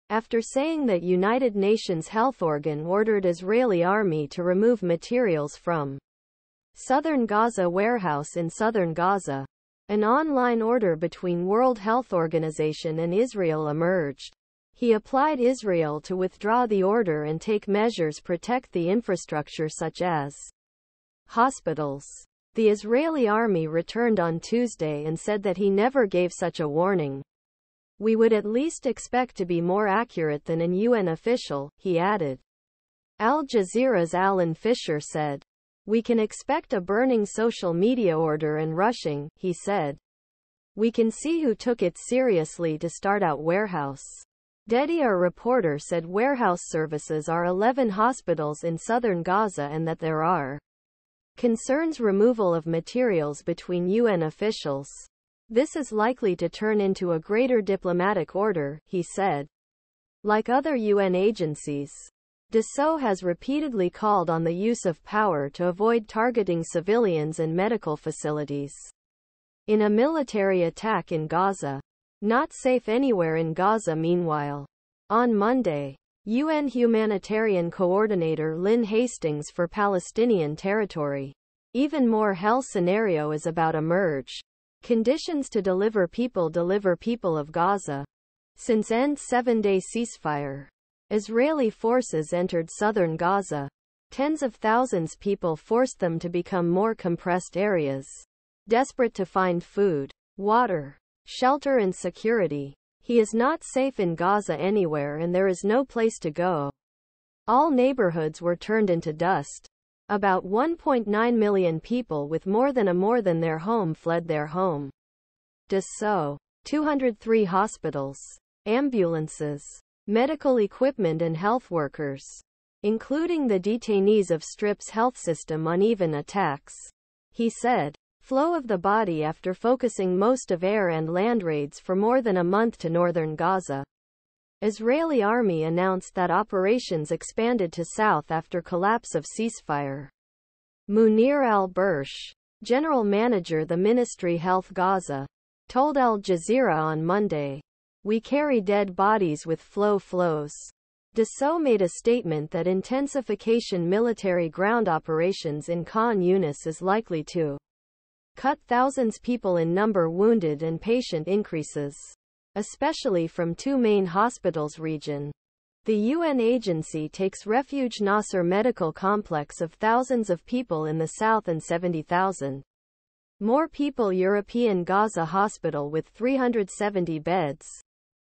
Israel and WHO in online row over removal of medical supplies in Gaza
After saying that United Nations Health organ ordered Israeli army to remove materials from Southern Gaza warehouse in Southern Gaza, an online order between World Health Organization and Israel emerged.  He applied Israel to withdraw the order and take measures protect the infrastructure such as hospitals. The Israeli army returned on Tuesday and said that he never gave such a warning. “We would at least expect to be more accurate than an UN official,” he added. Al Jazeera’s Alan Fisher said, “We can expect a burning social media order and rushing,” he said. “We can see who took it seriously to start out warehouse, dedi our reporter said warehouse services are 11 hospitals in Southern Gaza and that there are concerns removal of materials between UN officials. “This is likely to turn into a greater diplomatic order,” he said. Like other UN agencies, DSÖ has repeatedly called on the use of power to avoid targeting civilians and medical facilities in a military attack in Gaza. "Not safe anywhere in Gaza" Meanwhile, on Monday, UN Humanitarian Coordinator Lynn Hastings for Palestinian territory, “Even more hell scenario is about emerge, conditions to deliver people deliver people of Gaza”. Since end seven -day ceasefire, Israeli forces entered Southern Gaza, "tens of thousands people" forced them to become more compressed areas, desperate to find food, water, shelter and security. "He is not safe in Gaza anywhere andThere is no place to go. " All neighborhoods were turned into dust;About 1.9 million people with more than a more than their home fled their home. DSÖ, 203 Hospitals, ambulances, medical equipment and health workers, including the detainees of Strip's health system uneven attacks, he said. "Flow of the body" After focusing most of air and land raids for more than a month to Northern Gaza, Israeli army announced that operations expanded to south after collapse of ceasefire. Munir Al-Bursh, General Manager the Ministry Health Gaza, told Al Jazeera on Monday, "We carry dead bodies with flow flows." DSÖ made a statement that intensification military ground operations in Khan Younis is likely to cut thousands people in number wounded and patient increases, especially from two main hospitals region ”. The UN Agency takes refuge Nasser Medical Complex of thousands of people in the south and 70,000 more people European Gaza Hospital with 370 beds.